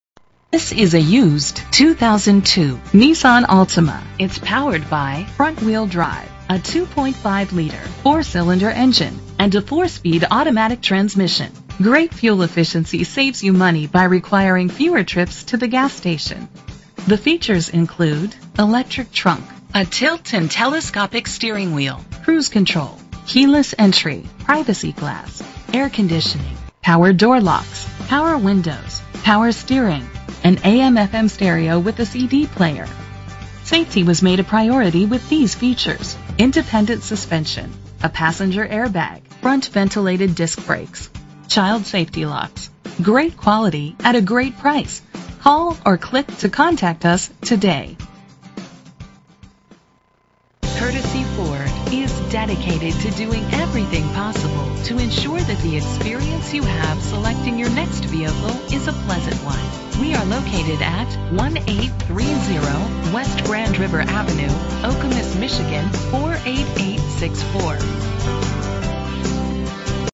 2002 Nissan Altima - Okemos MI
Phone:
Year: 2002
Make: Nissan
Model: Altima
Trim: 2.5 S
Engine: 2.5 liter inline 4 cylinder 16 valve
Transmission: 4-Speed Automatic
Color: Velvet Beige
Mileage: 119755
Address:
1830 West Grand River Ave.